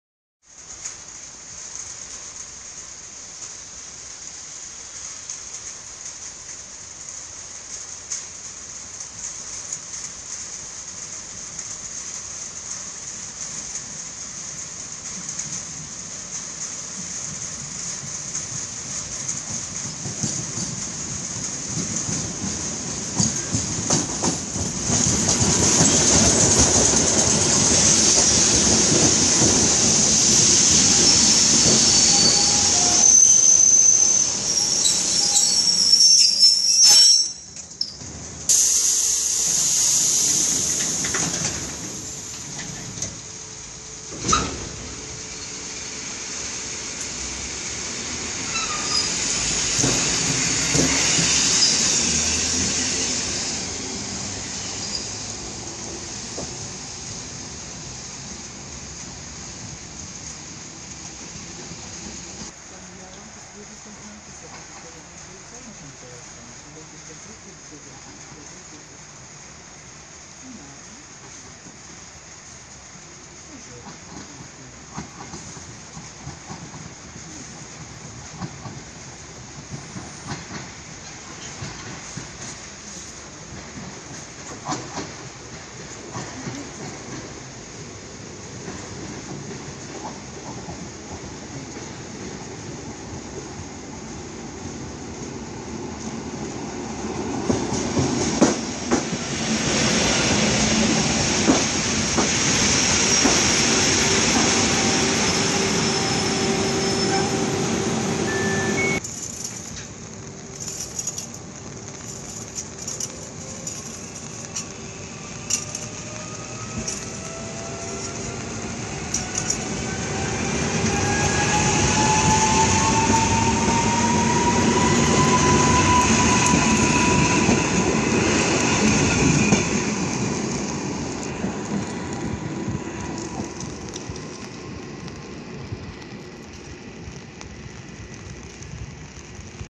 Deszczowy wieczór na stacji PKP Chrzanów.
1.EN57-1170 relacji Kraków Główny-Oświęcim ten sam co w poprzednim filmiku😅
2.EN57FPS-1583 relacji Czechowice Dziedzice-Kraków Główny
Data nagrania 25.02
Życzę miłego dnia😎